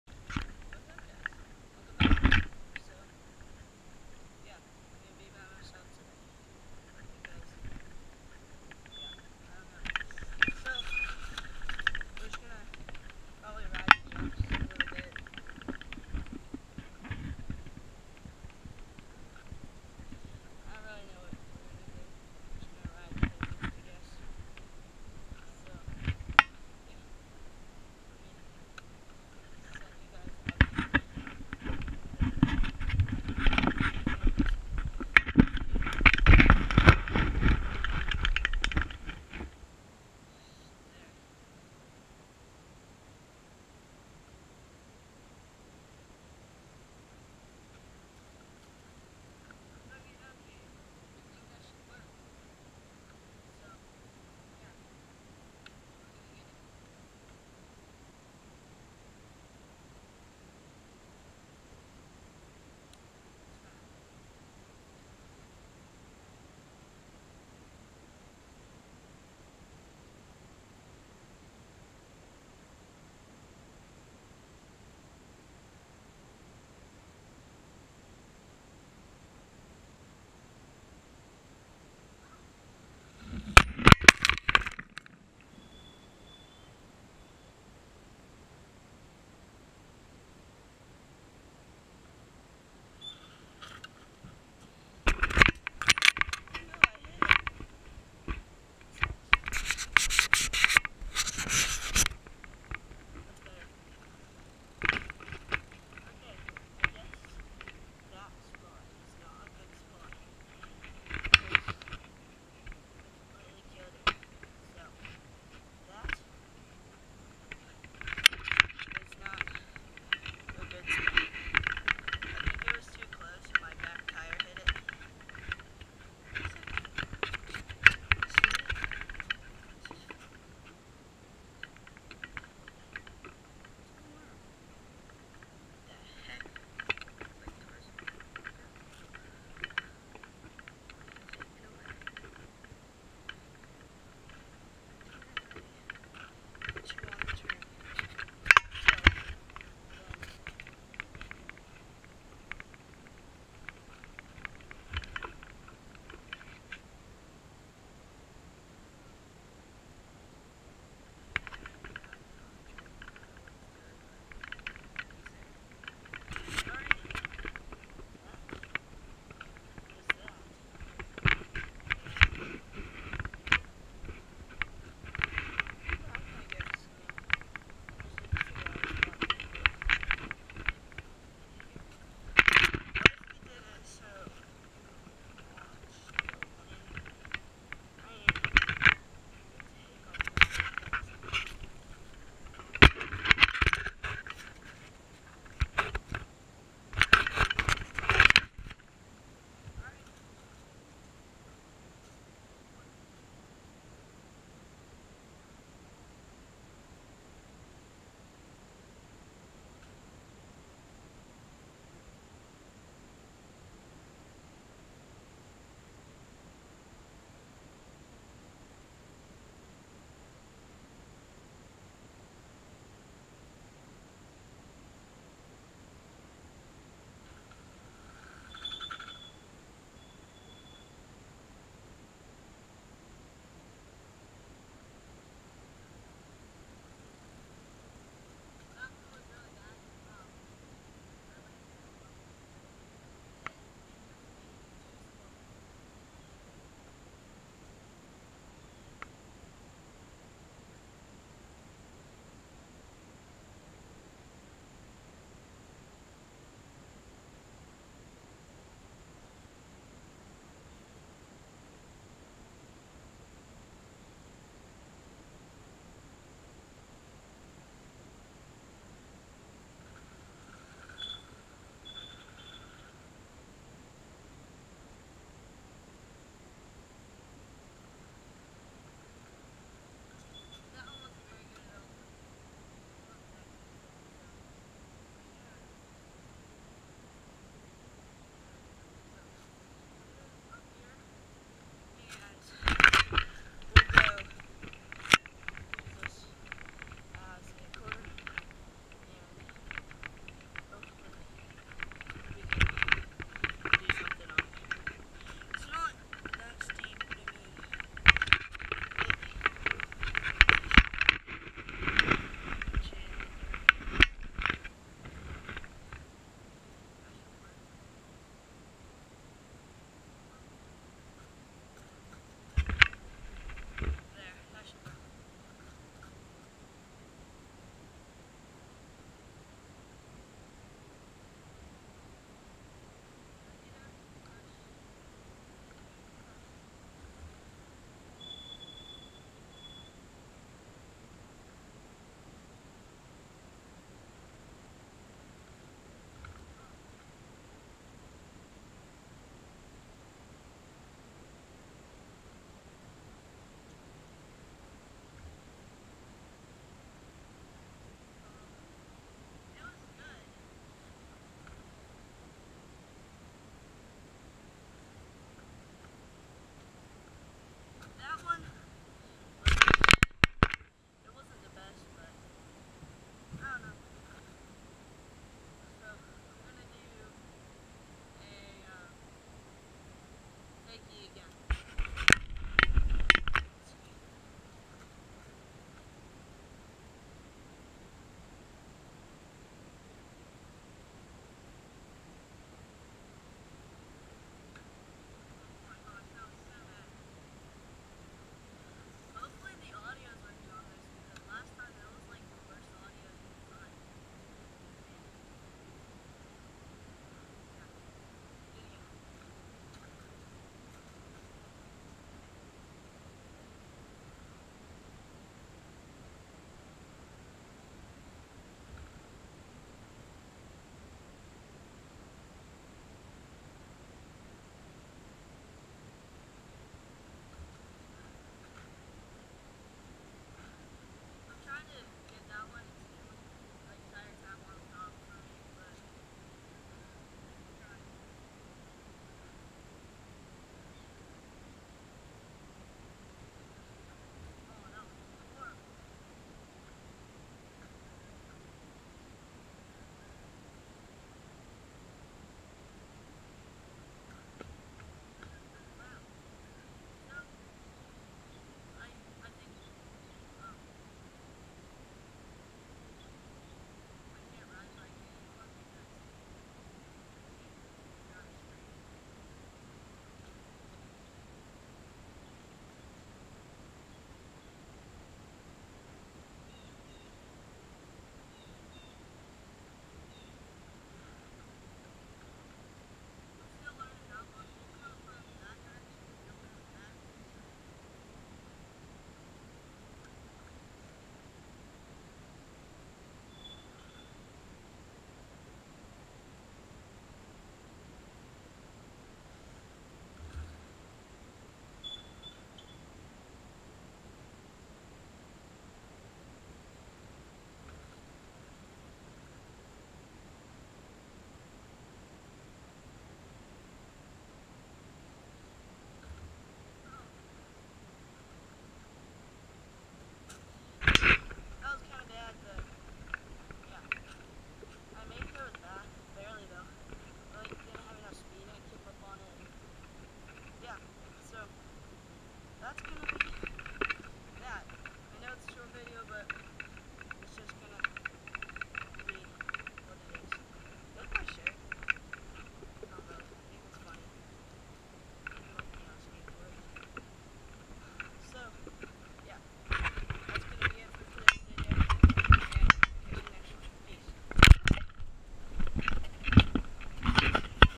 Hitting the jumps! (Riding by myself)
Hitting the big table top, flying high, having a good time! Buuuuuuuuuuuuuuuttttt sadly I am riding by myself! But still overall i still had a great time!!

All Ethan's action films
 You won't regret it I promise.